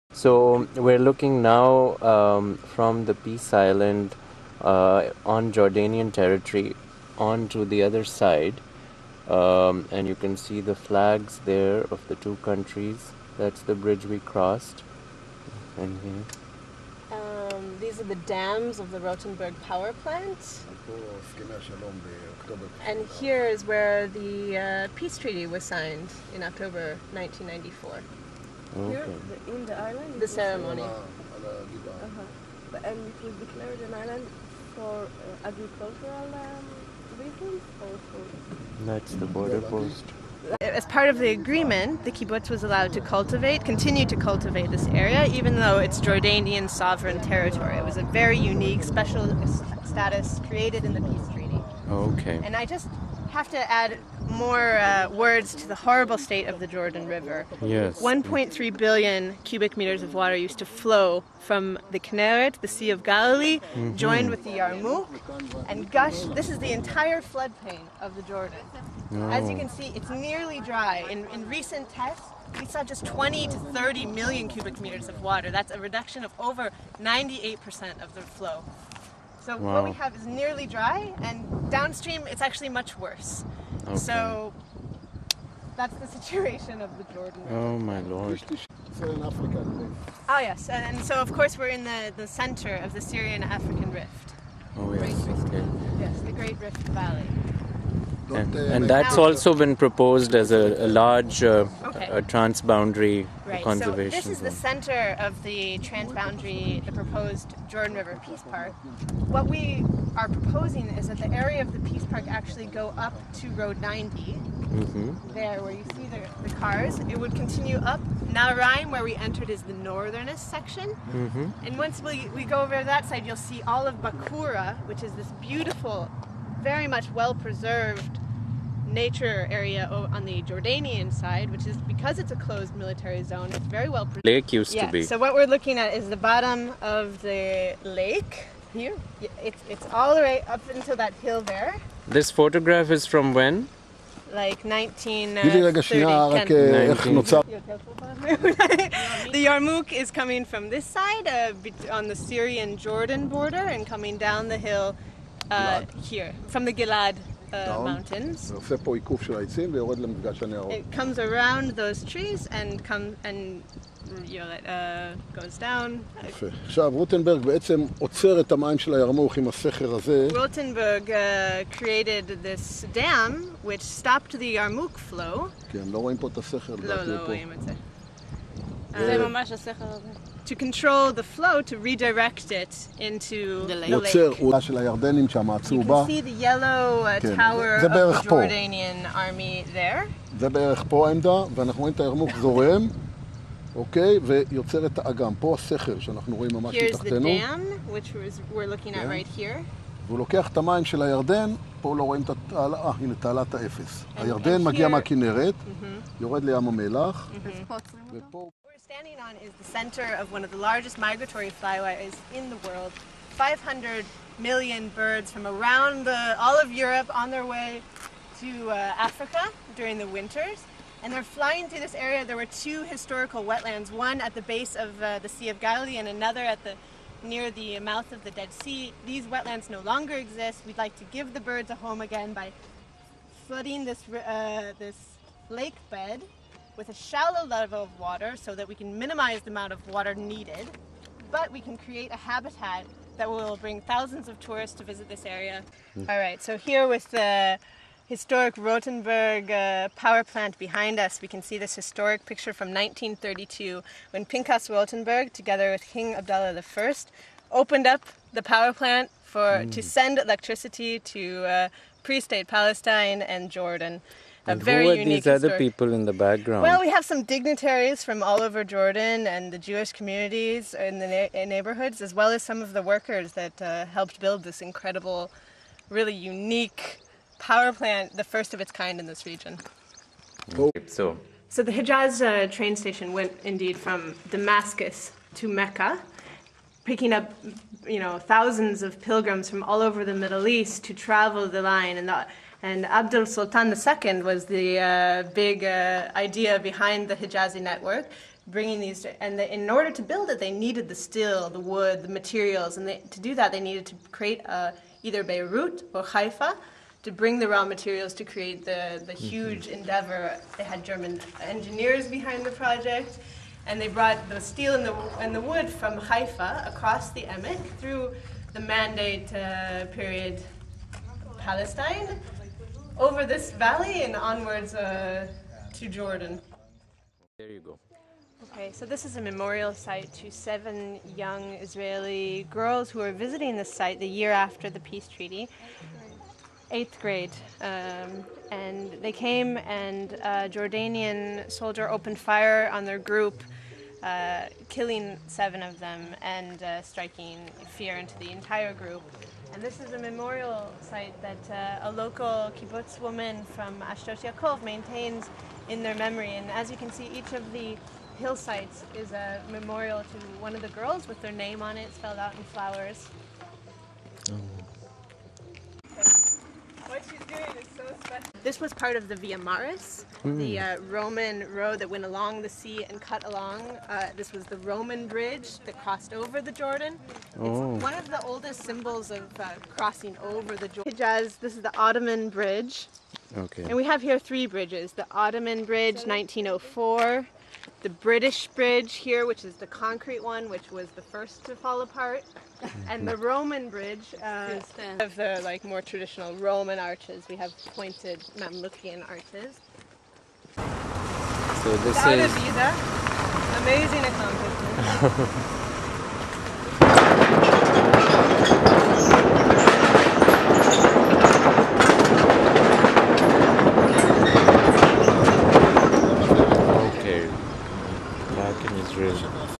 Jordan Israel Environmental Peace Park -- Saleem Ali
A visit to the peace "island" between Jordan and Israel which Friends of the Earth -- Middle East is working on converting into an international peace park. Filmed by Saleem H. Ali (University of Vermont, editor of the MIT Press book "Peace Parks: Conservation and Conflict Resolution) with commentary by Elizabeth Ya'ari (Friends of the Earth Middle-East), January 2010.